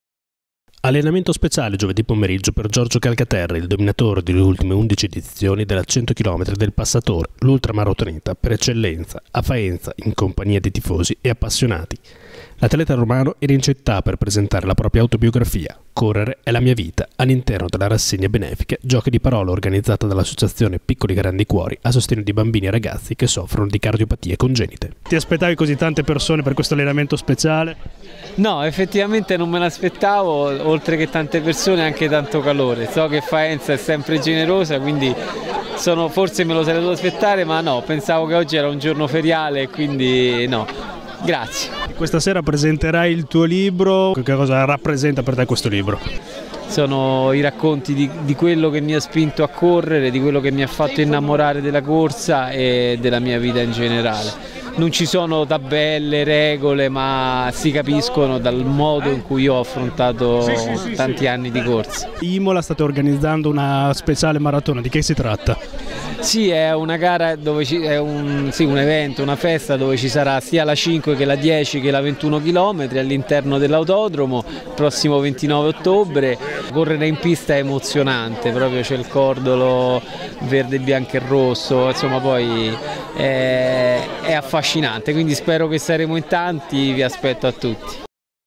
FAENZA: Allenamento coi tifosi per Calcaterra in vista della 100 Km | VIDEO
Allenamento speciale a Faenza per Giorgio Calcaterra, il dominatore delle ultime 11 edizioni della 100 Km del Passatore, l’ultramaratoneta per eccellenza, in città in compagnia di tifosi e appassionati. L’atleta romano era in città per presentare la propria autobiografia, “Correre è la mia vita”, all’interno della rassegna benefica “Giochi di Parole”, organizzata dall’associazione “Piccoli Grandi Cuori” a sostegno di bambini e ragazzi che soffrono di cardiopatie congenite.